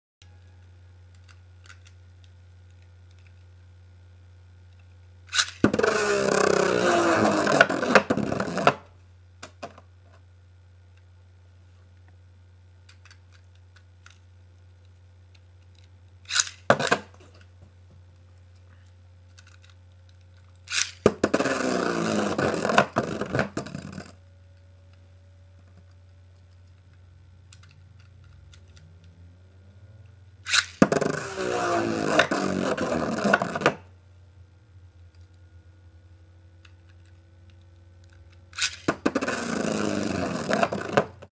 For When You're Bored
If you have nothing better to do, try out this cool Beyblade trick.